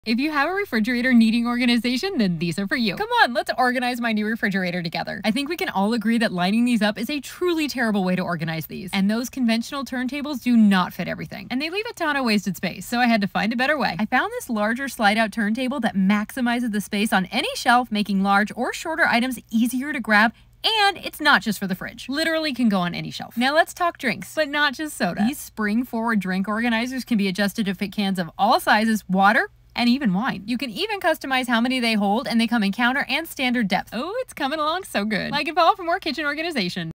TWO better & EASY ways to organize a refrigerator! Part one of the kitchen organization series!
these two organization products have helped make my refrigerator more functional!

I think we all can agree lining up condiments bottles is a truly terrible way to store these (so hard to see what you have)… And conventional turntables don’t fit everything and leave too much wasted space

I found this larger slide out turntable by that maximizes the space on any shelf making large or shorter items easier to grab. And it’s not just for the fridge. I love you!

Let’s talk drinks! But not just soda. These spring forward drink organizers can be adjusted to fit cans of all sizes, water, and even wine!

(They come in counter depth and standard depth)

I hope you love them too!!